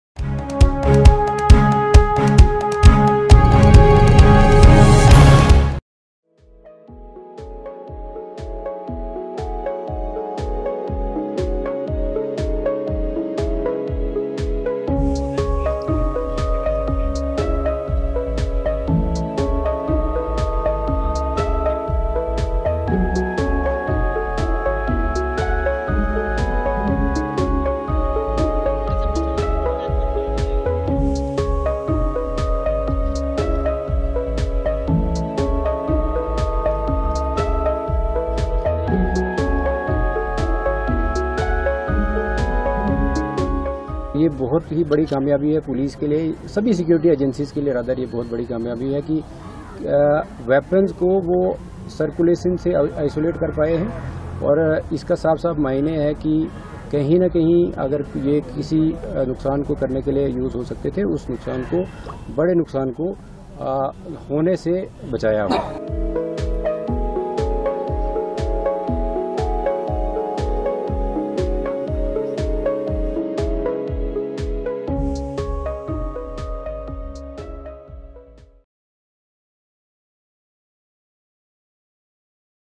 J&K police busts terrorist hideout in Rajouri, recovers huge cache of weapons
In a major counter insurgency operation, Jammu and Kashmir (J&K) Police on Wednesday busted a hideout of terrorists in Chatyari forest area of Rajouri district and recovered a stockpile of weapons. The seized ammunition included four automatic assault rifles, four AK rifles, 11 pistols and sets of magazines. Calling it a huge achievement for the security agencies, Rajouri SSP Yougal Manhas said that the removing such a stockpile of weapons from circulation would help the forces in limiting future attacks.